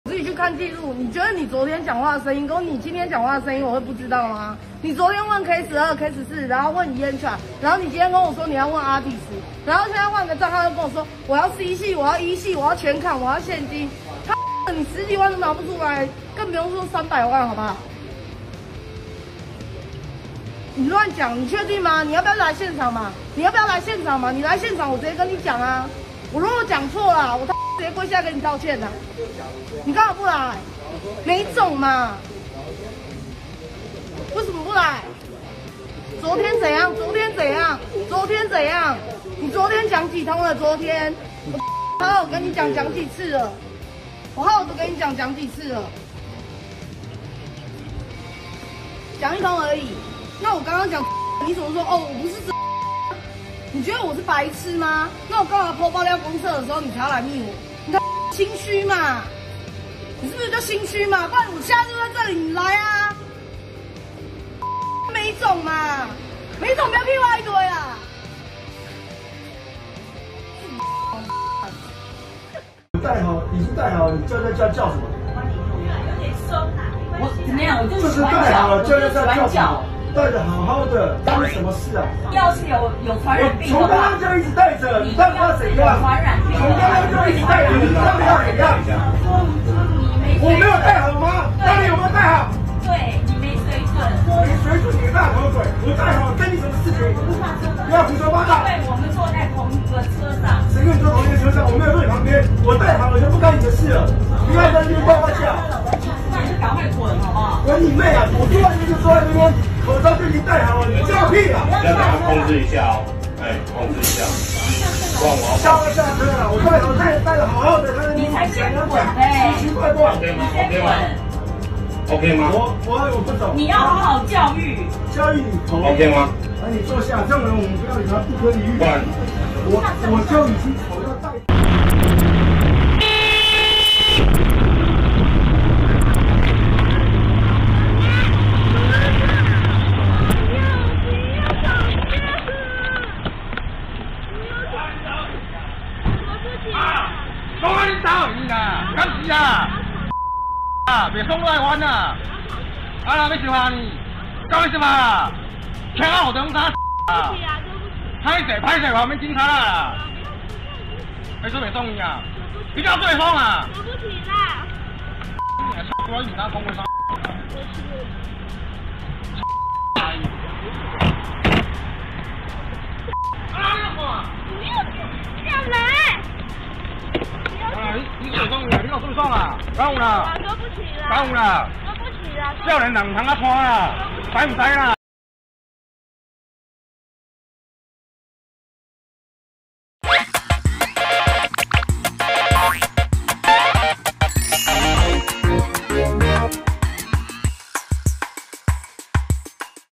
客人藉買車之名狂騷擾！女業務怒嗆：有種就來｜車行｜糾紛｜社會
☑影片授權：FB/奕心中古車專賣